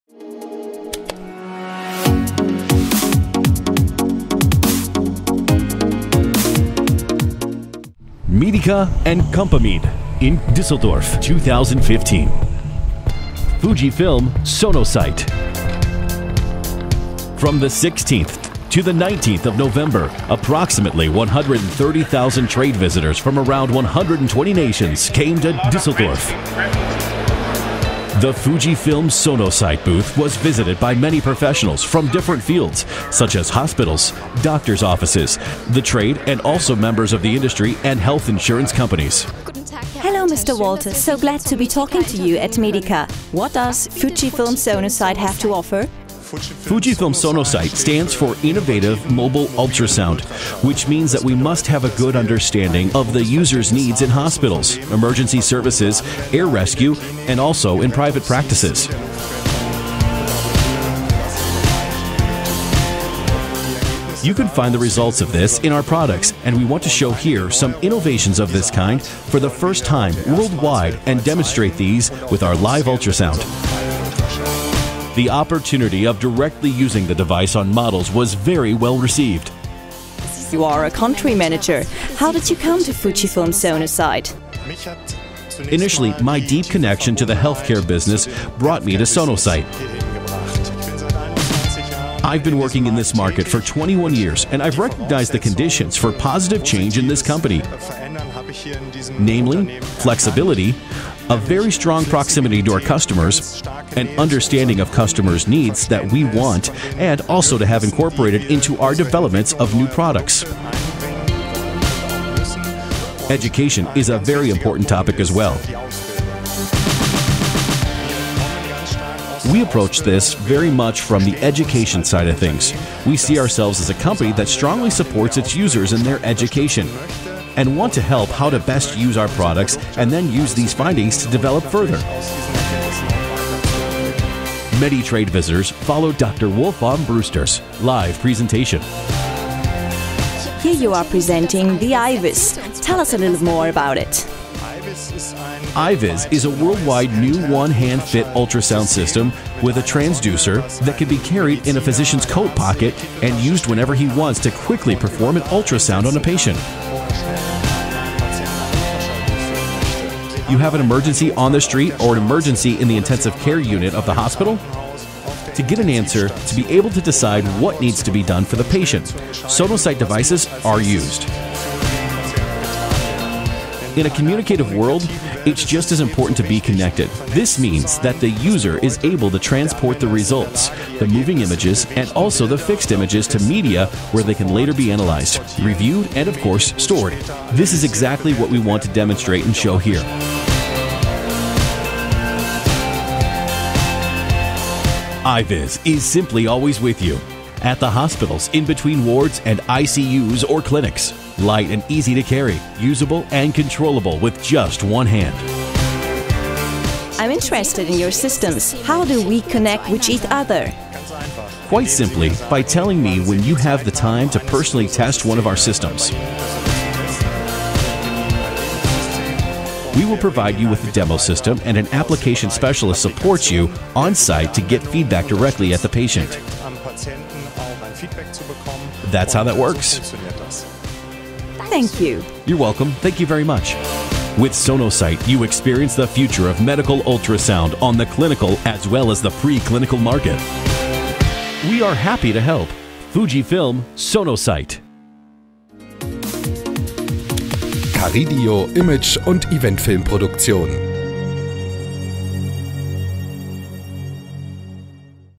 FUJIFILM SonoSite GmbH with the iViz Ultrasound system at Medica
FUJIFILM SonoSite with iViz ultrasound system - Medica - Karrideo.

for more videos like this.